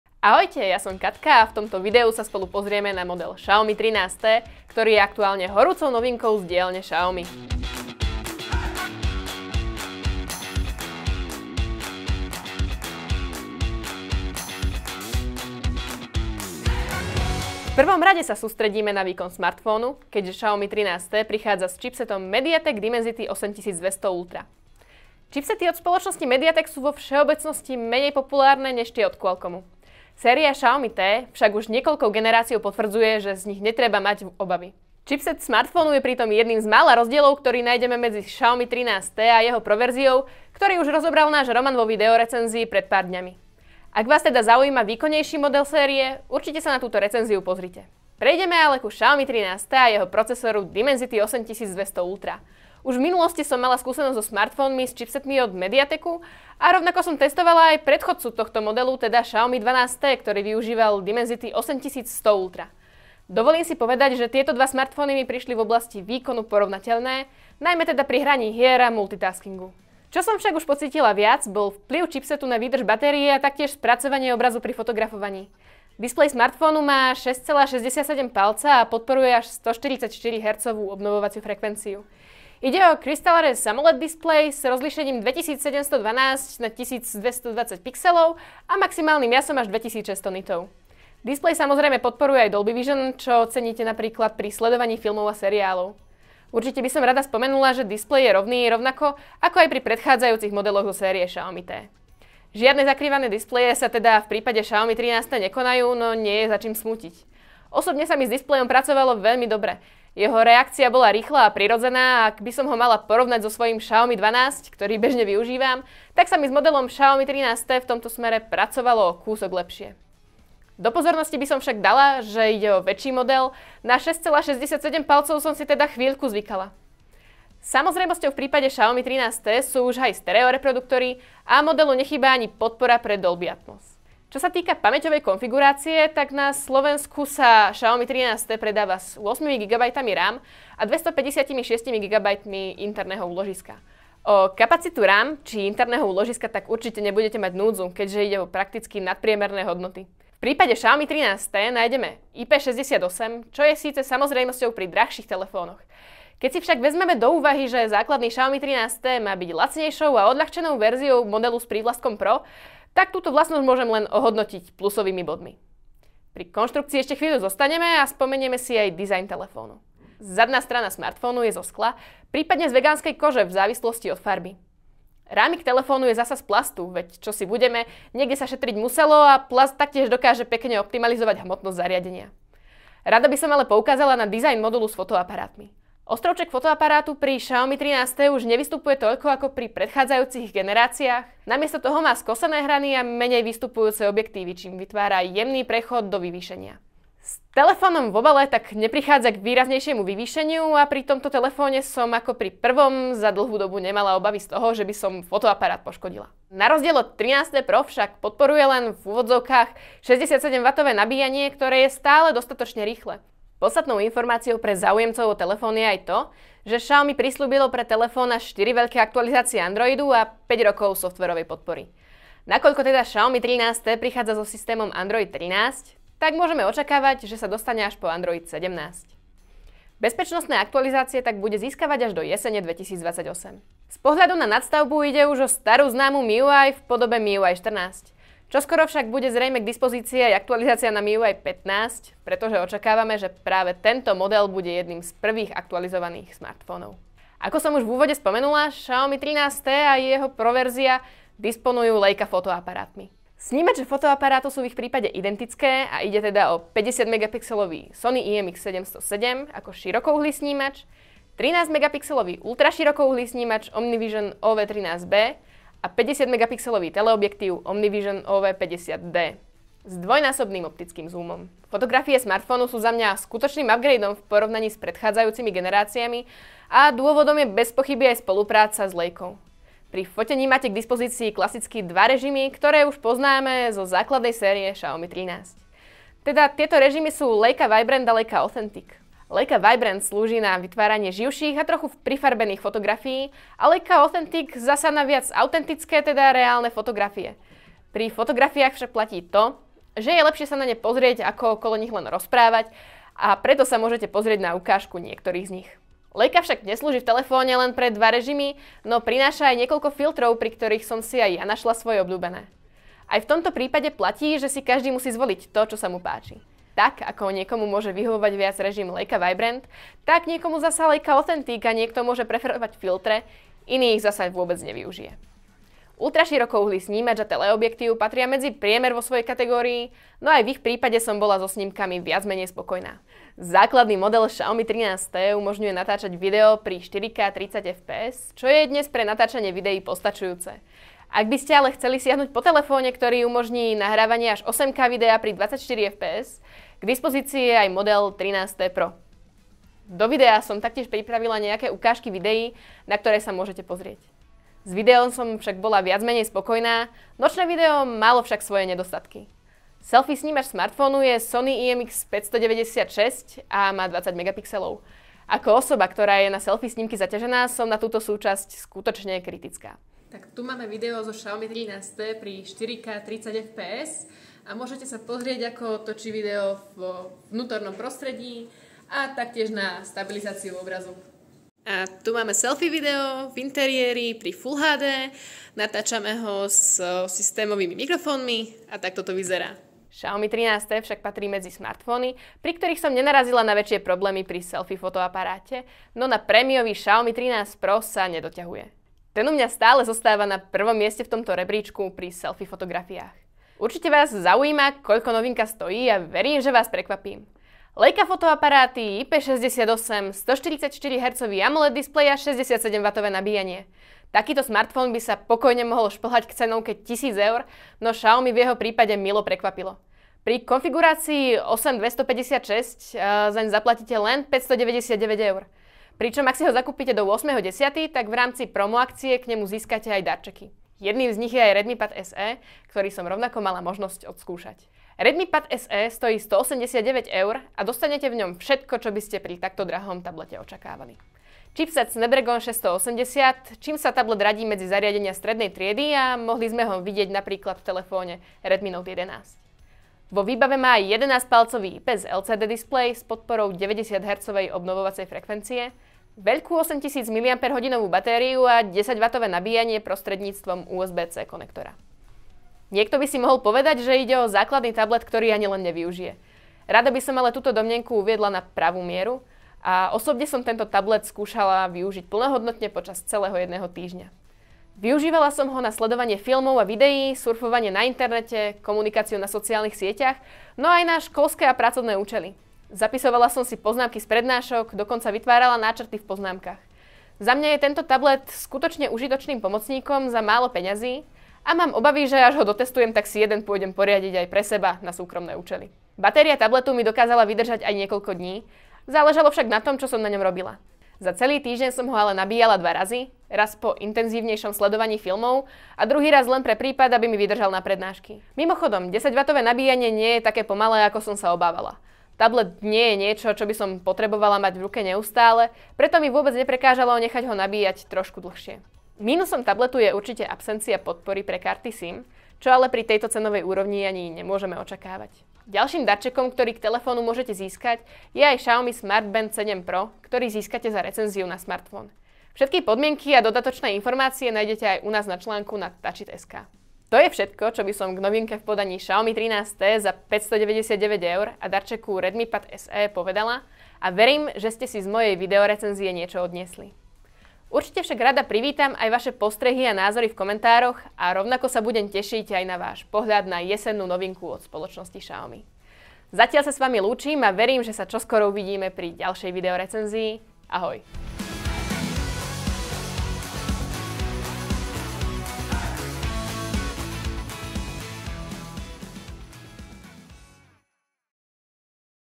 Xiaomi 13T: Výhodný smartfón s Leica fotoaparátmi (RECENZIA)
Xiaomi 13T prichádza s čipsetom MediaTek Dimensity 8200-Ultra, trojicou fotoaparátov s optikou od Leicy aj certifikáciou IP68. Vo výbave ďalej ponúka aj 6,67-palcový AMOLED displej so 144 Hz obnovovacou frekvenciou či 67 W nabíjanie 5000 mAh batérie. Ako však Xiaomi 13T funguje v praxi som zhrnula do tohto videa.